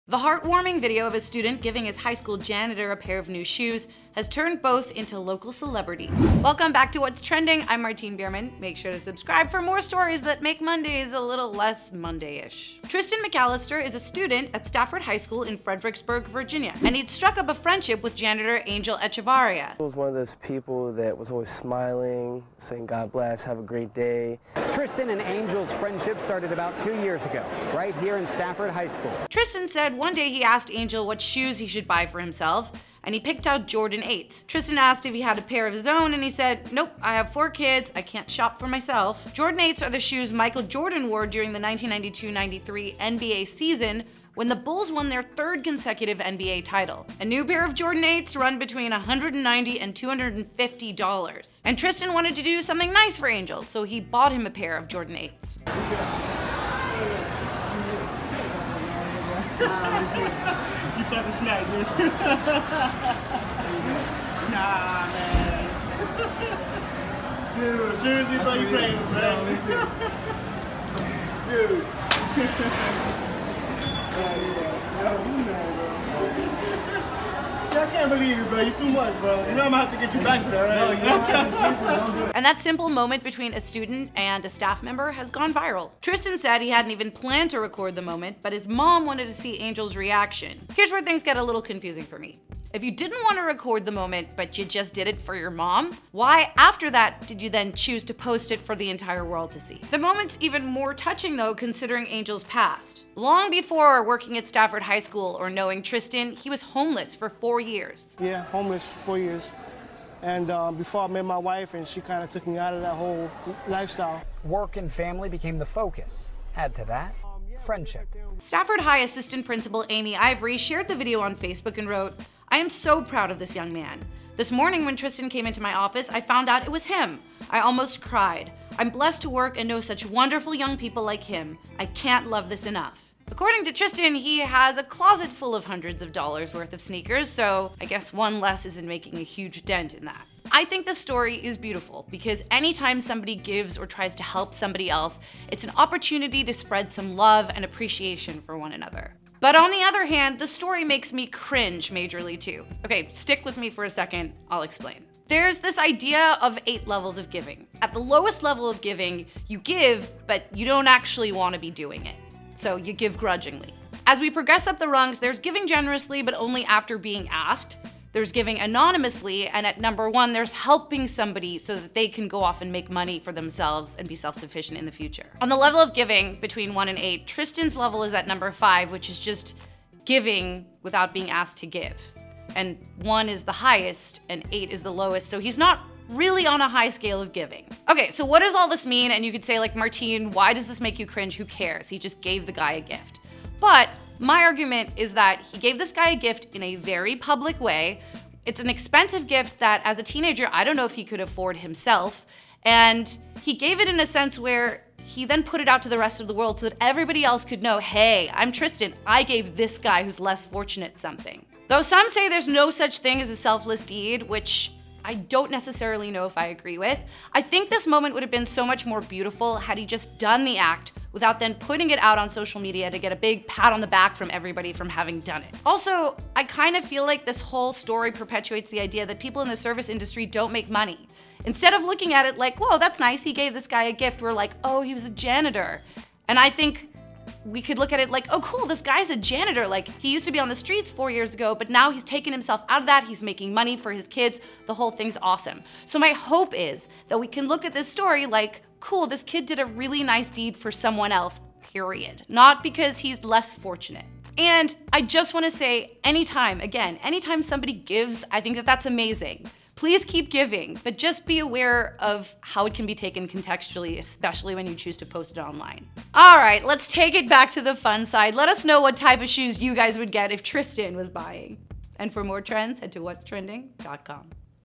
High School Student Gives Jordans to Janitor in Heartwarming Viral Video
The heartwarming video of Stafford High School student Tristan McAlister giving janitor Angel Echevarria a pair of new Jordan 8’s has turned both into local celebrities.